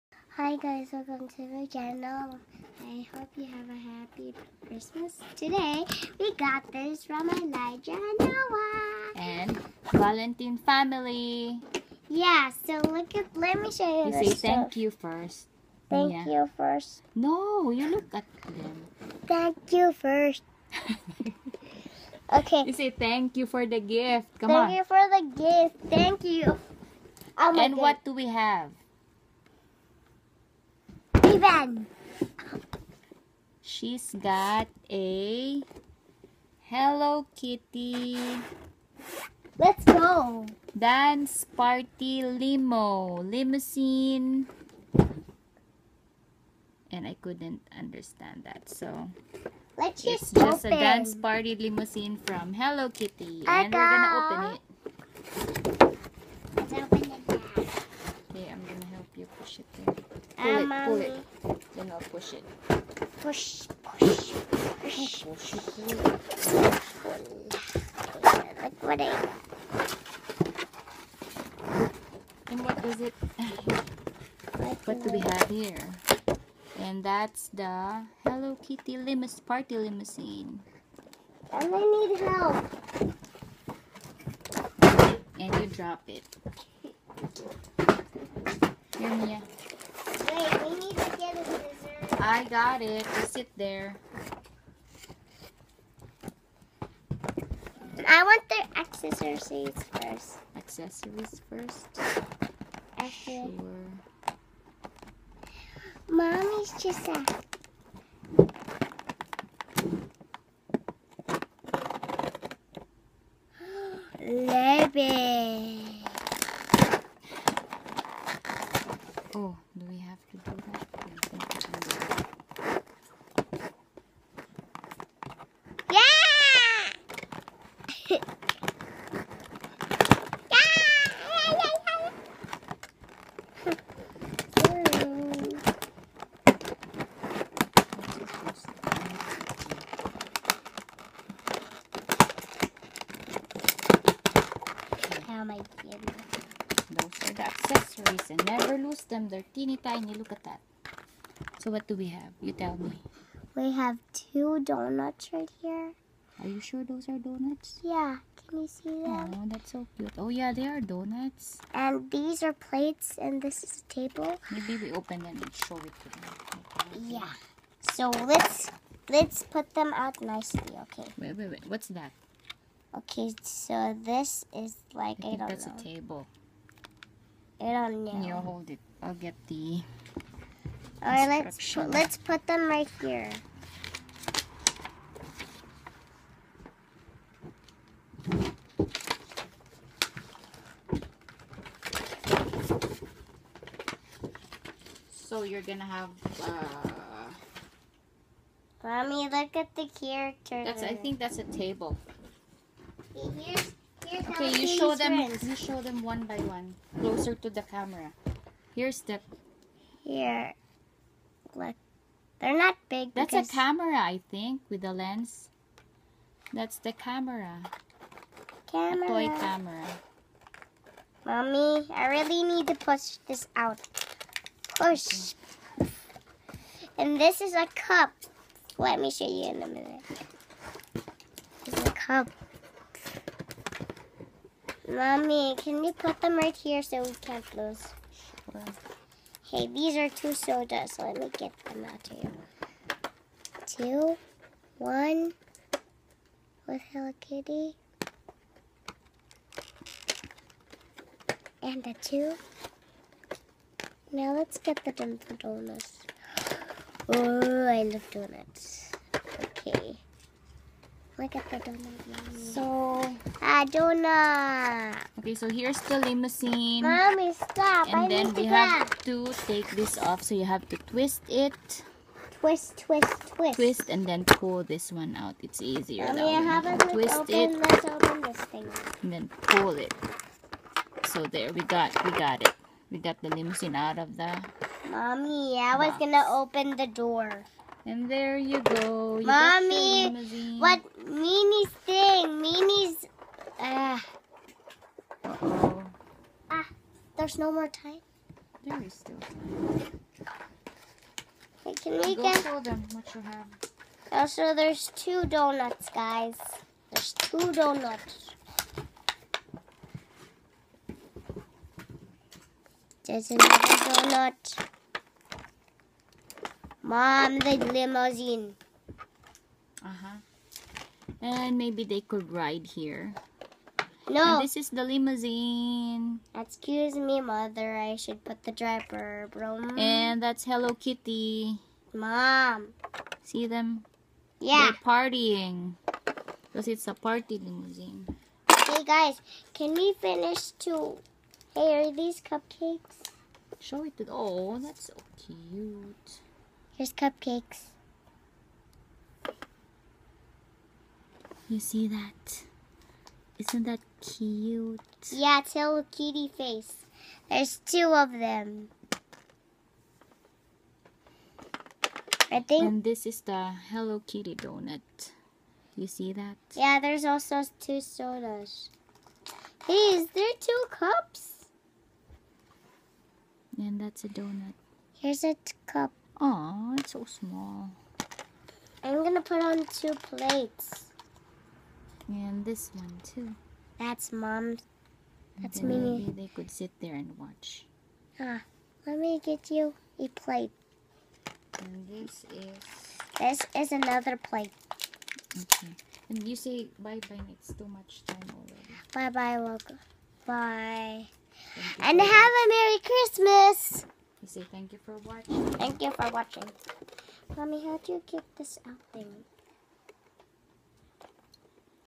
Hello Kitty (Dancing Party Limo)
Don't mind my unkempt nails. That's a hardworking and hands-on, Mom's hand you're looking at. Lol. Thanks for the gift Valentine Family. We surely love it.